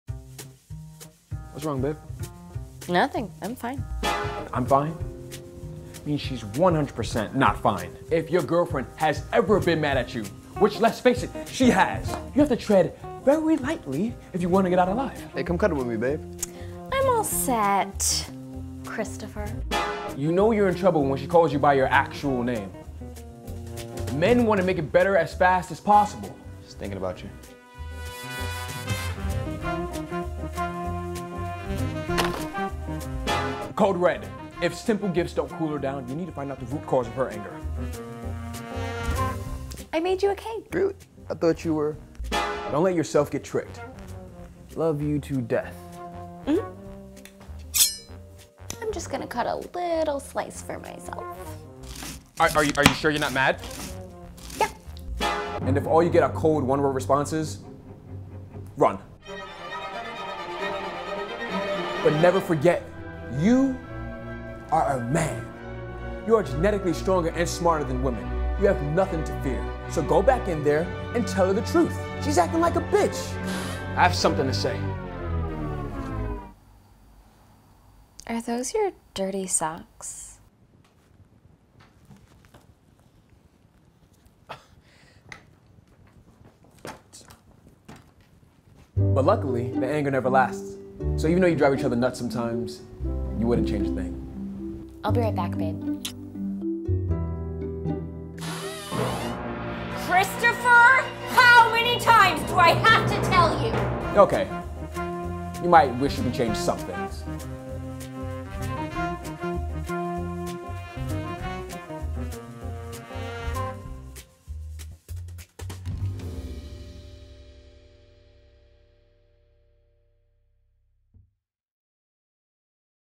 Relationship Advice: How to Escape Bae's Rage When She's Mad | CoupleThing
Have you ever been mad at your bf and wanted nothing to do with him for a couple of days? Same. You've probably felt angry, mad and enraged all at the same time. He might try to buy you some flowers after a fight, but that doesn't solve anything! Here's some advice, just accept that you're in hot water for now and the anger will eventually go away. We don't like to be bribed!